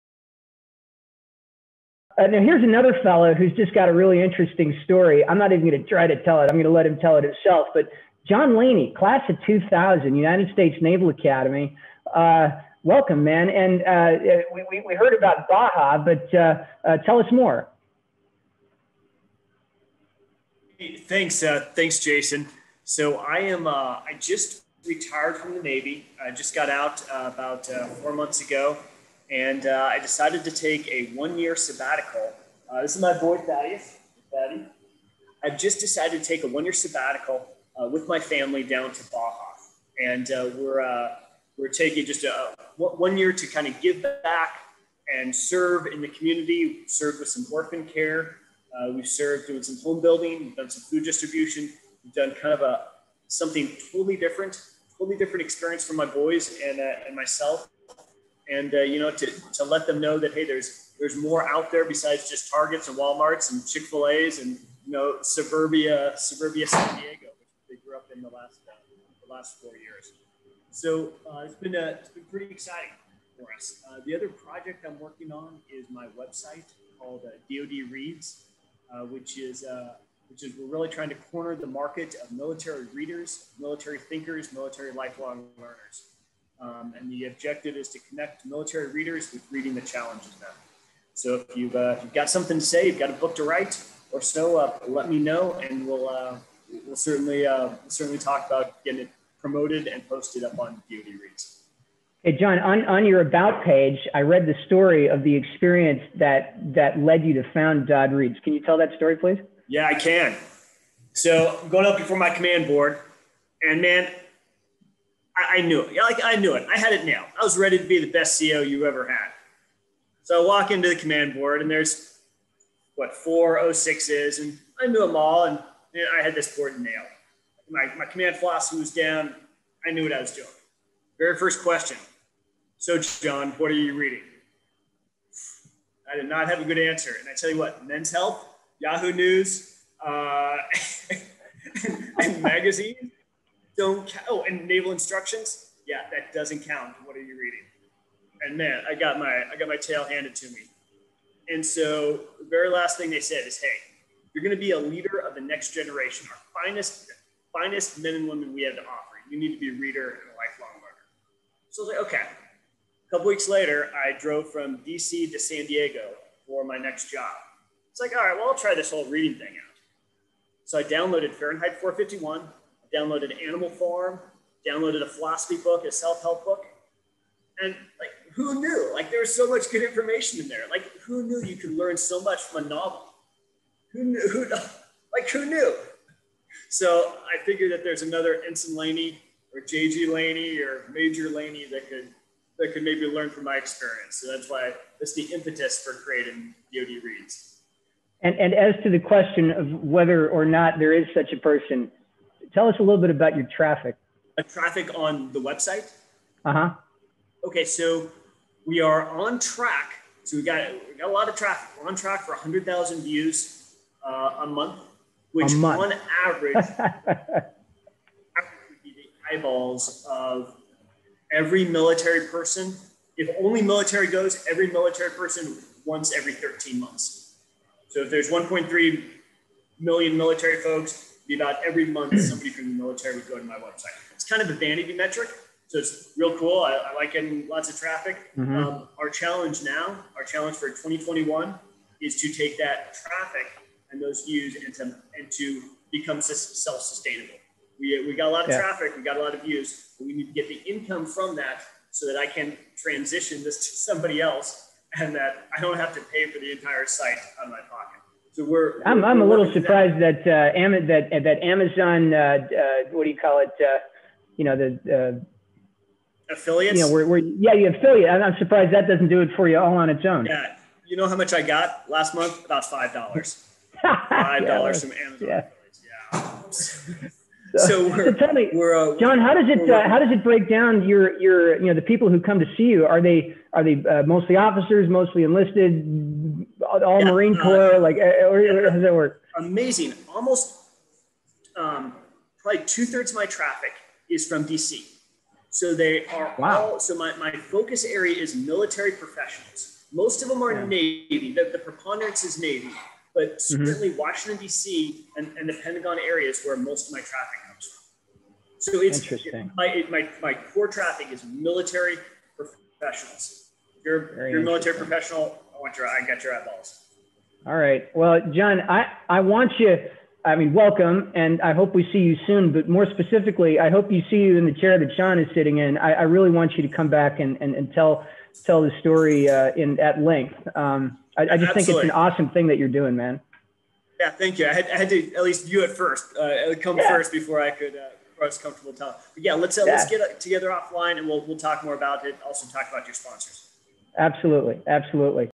2020-11-19 RINGKNOCKER Virtual Meetup - John Laney on DODReads
John Laney (USNA '00) is the founder of a website that connects military readers with great stuff to read! DODReads is HUGE... they currently log over 100,000 page views a month and are still growing! In this clip, John shares a bit of his own story, including the experience that prompted him to create the website in the first place.

Are you a current or former United States Military officer? Join a RINGKNOCKER Virtual Meetup!